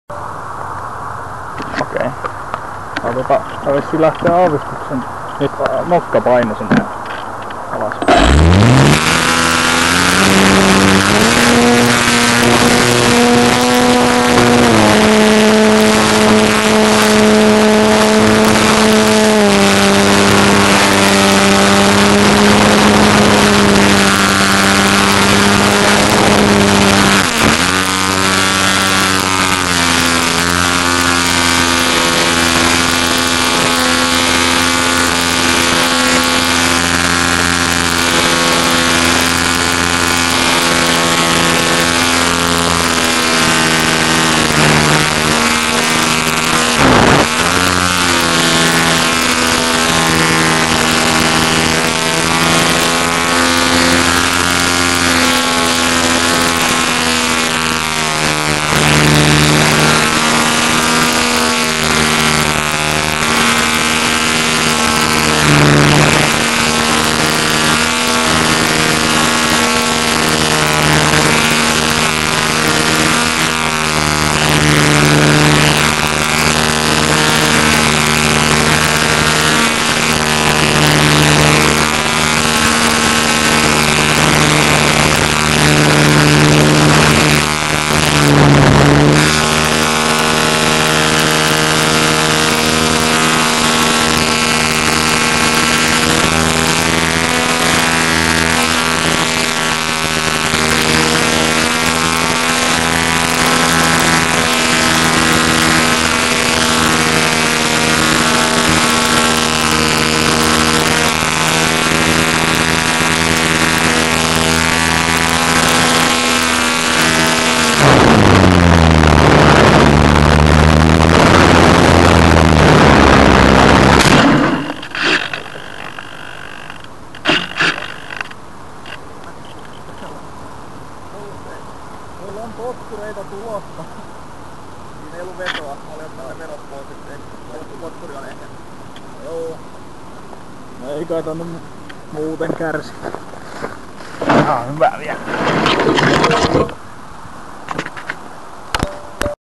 Skyartec Cessna 182, "Mercedes" keycam ja rauhallista lentoa
Lentäjän vaihduttua näkee hyvin, kuinka hyvin Skyartec Cessna 182 lentää osaavissa käsissä hitaasti lähellä maata.

Tämä pätkä on sen jälkeen, kun hain koneen pöpeliköstä edellisen lentoni jäljiltä, josta ei tullut vahinkoja.